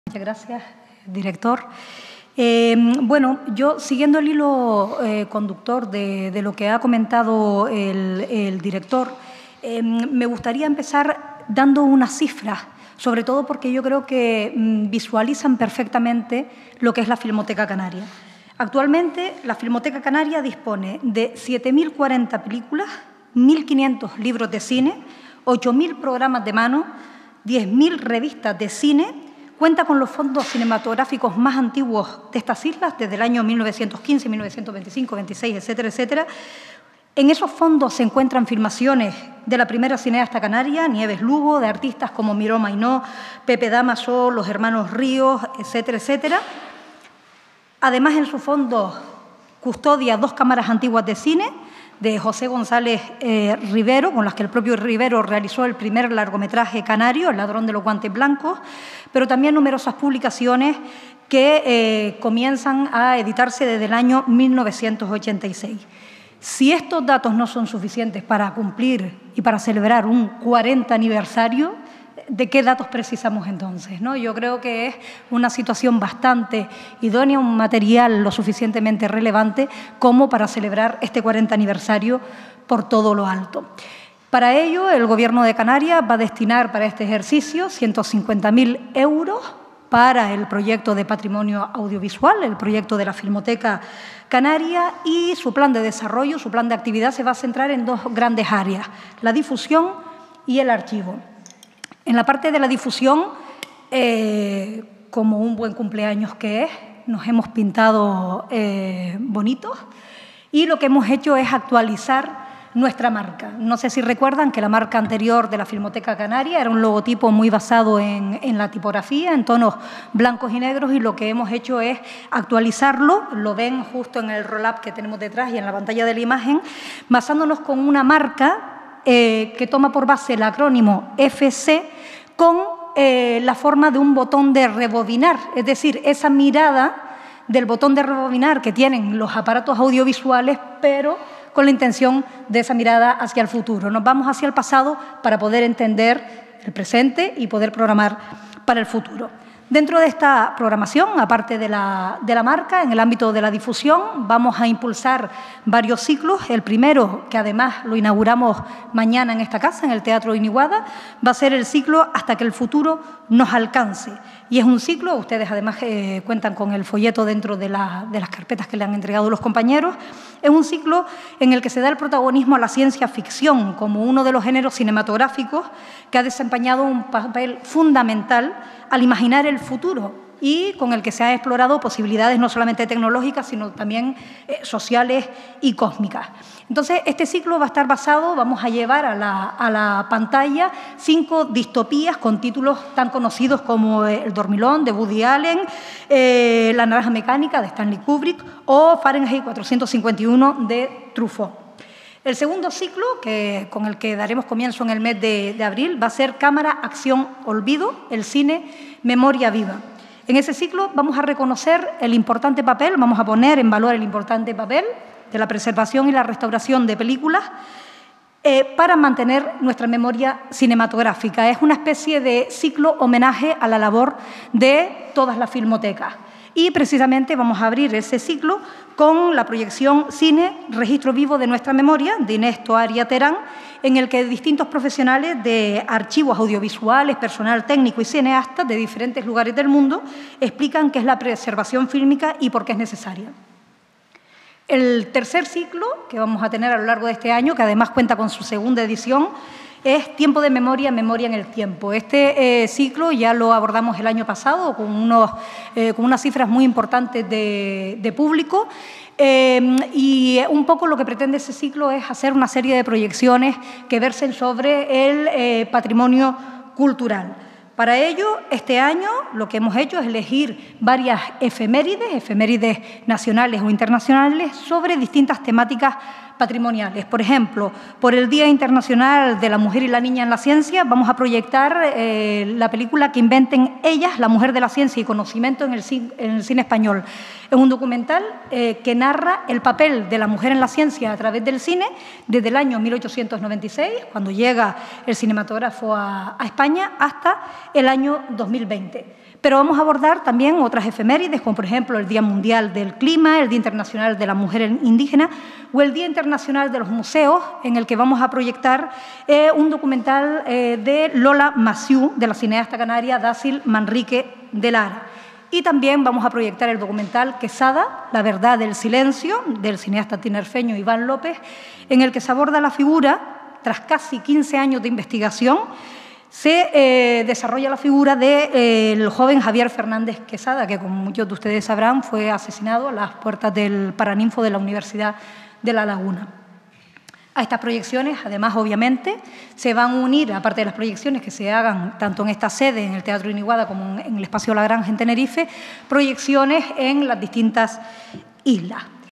Filmoteca Canaria cumple 40 años con un amplio programa de actividades
El 3 de noviembre de 2024, la Filmoteca Canaria cumple 40 años. Para celebrar su trayectoria, el Gobierno, a través de la Dirección General de Cultura y Patrimonio Cultural y del Instituto Canario de Desarrollo Cultural (ICDC), organiza un calendario de iniciativas que en líneas generales buscan trasladar la importancia y el reconocimiento de la conservación y difusión del legado audiovisual canario.

En la mañana de hoy, el director general de Cultura y Patrimonio Cultural, Miguel Ángel Clavijo, y la coordinadora de la Unidad de Patrimonio del Instituto Canario de Desarrollo Cultural (ICDC), Mari Carmen Naranjo, presentaron a los medios de comunicación las nuevas líneas de trabajo de Filmoteca Canaria.

“A partir de ahora el Gobierno va a hacer un extraordinario esfuerzo en divulgar lo que se ha hecho en estos últimos cuarenta años, lo que queda por hacer y paralelamente a estas dos líneas de trabajo, a exponerle a la población todo lo que tenemos en el archivo”, asegura Clavijo. Se trata de una buena oportunidad “para renovarse” pero también para insistir en la labor de “proteger todo lo que tiene que ver con el patrimonio audiovisual de Canarias”, concluye.

Para Naranjo, es fundamental trabajar en la idea de la “Filmoteca fuera de la Filmoteca” porque hasta ahora “se ha trabajado, en gran medida, para la conservación y creación de sus fondos y toda esa ingente masa documental con la que cuenta queremos hacerla pública”. En realidad eso va a ser “uno de los grandes retos de este proyecto, poner en uso público los fondos de la Filmoteca, no solamente para este año sino durante los años venideros”, adelanta.

Los archivos audiovisuales nos cuentan historias sobre la vida de las personas y las culturas. Representan un patrimonio inestimable y una afirmación de nuestra memoria colectiva, así como una valiosa fuente de conocimientos, ya que reflejan la diversidad cultural, social y lingüística de nuestras comunidades. Conservar este patrimonio y asegurar que permanezca accesible al público y a las generaciones futuras es un objetivo vital para todas las instituciones dedicadas a la memoria. Por ello, el área que dirige Miguel Ángel Clavijo, busca dar un nuevo impulso a la cinemateca canaria a través de un programa de actuación que contempla la difusión y el propio archivo que atesora este centro.

Difusión

Una de las principales funciones de la Filmoteca Canaria es la difusión del cine que no llega a las pantallas comerciales y en versión original. Los ciclos que programa Filmoteca se proyectan en Gran Canaria (Teatro Guiniguada) y Tenerife (Espacio La Granja), así como en otros  espacios de las islas en colaboración con los cabildos insulares o ayuntamientos. En este apartado, se programará en el primer semestre del año sesiones relacionadas directamente con la reflexión sobre el pasado y el papel que el cine y las filmotecas ocupan en esta tarea:

‘Hasta que el futuro nos alcance’ ocupará la cartelera de los meses de  febrero y marzo. La ciencia ficción ha desempeñado un papel crucial al imaginar el futuro, explorando posibilidades tecnológicas, sociales y cósmicas que capturan la imaginación de los espectadores. El ciclo reúne cinco distopías cinematográficas que nos sumergen en los más aspectos oscuros del futuro tal como sus creadores lo imaginaron hace 40, 50 o 60 años atrás, como “El dormilón” (1973) de Woody Allen; “Lemmy contra Alphaville” (1965) de Jean-Luc Godard; “La naranja mecánica” (1971) de Stanley Kubrick; “Fahrenheit 451” (1966) de François Truffaut  y “El hombre que cayó a la Tierra” (1976) de Nicolas Roeg. Estas grandes películas hoy de culto, continúan siendo fuentes de inspiración y debate, recordándonos que nuestras elecciones actuales pueden moldear el curso de la historia futura.

Fuente: ACFI